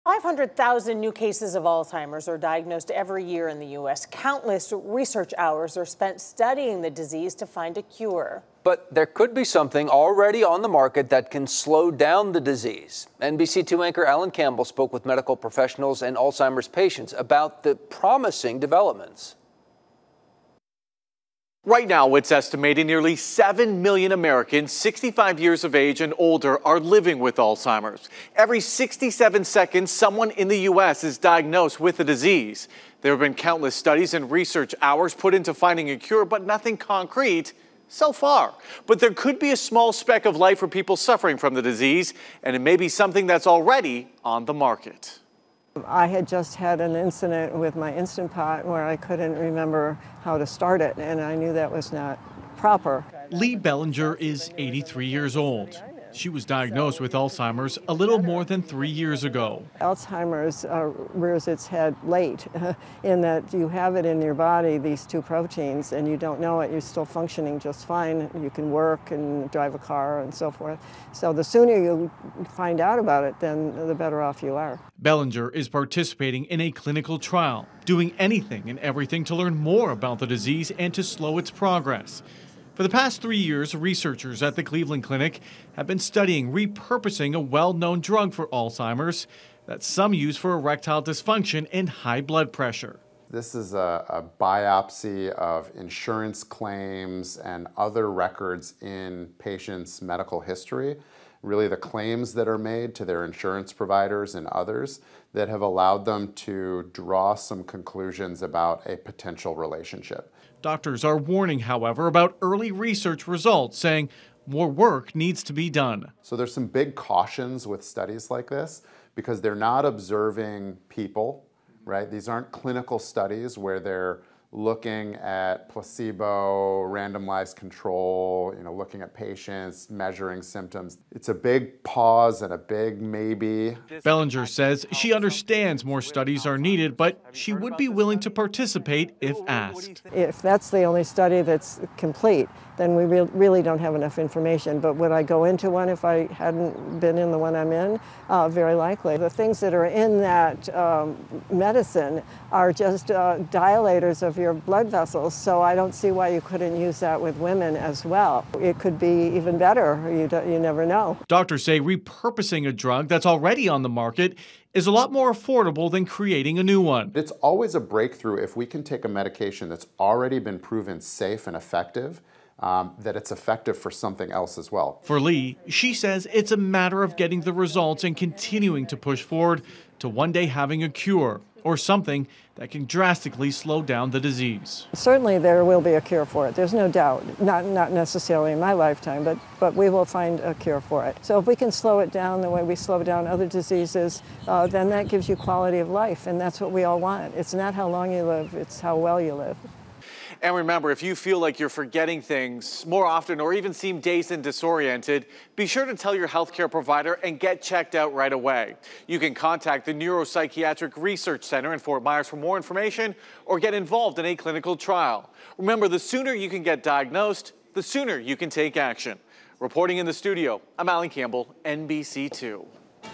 Researchers attempting to cure Alzheimer's by repurposing existing drug
Alzheimer's is a cruel and devastating disease. Nearly 500-thousand new cases are diagnosed every year in the U.S.